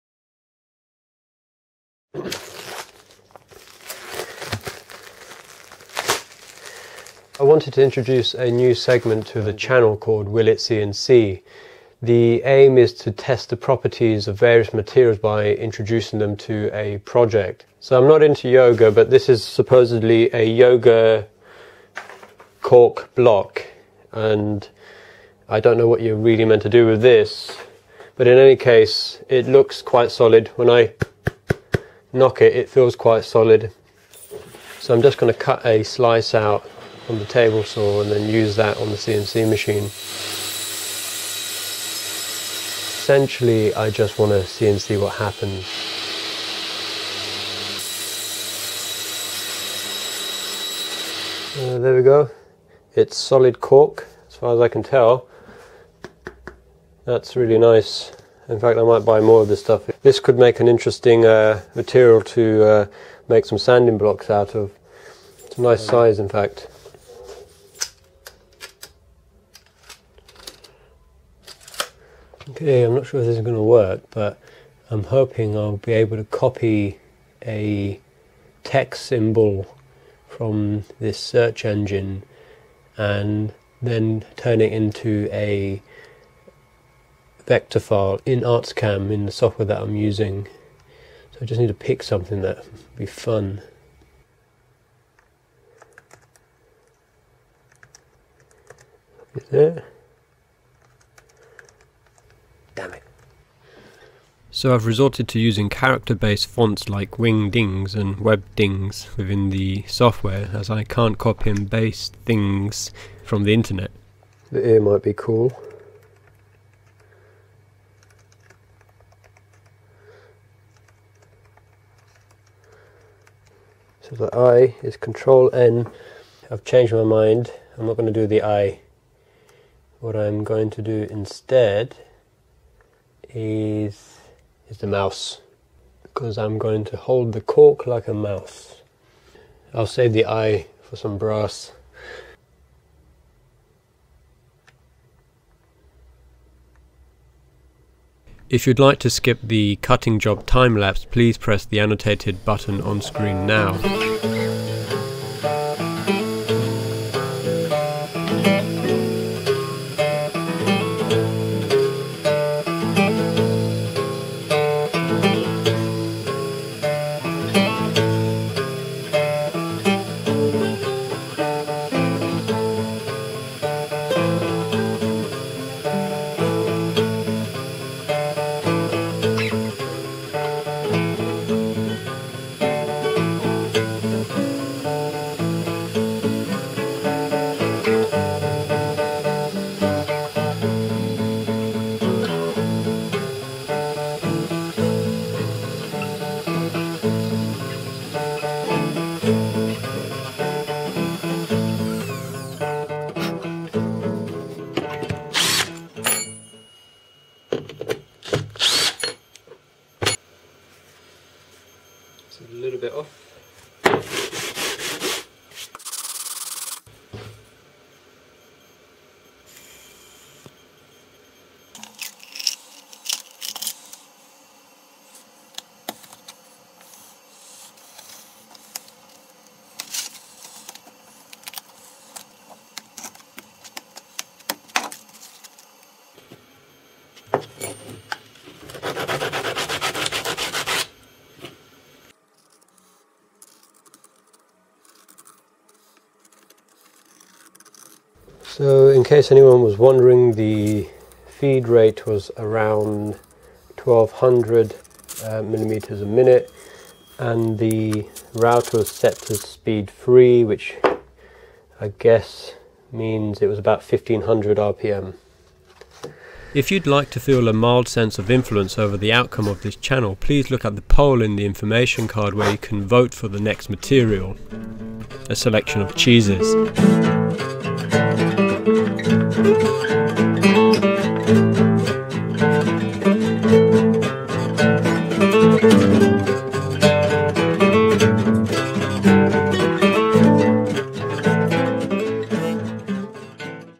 Will It CNC #1 Cork / Making a Sanding Block
00:02 Introducing will it CNC.
01:12 Preparing file.
02:33 Cutting timelapse.
03:28 Making a sanding block.
04:25 Pick the next material.

Poll will close when I get around to checking.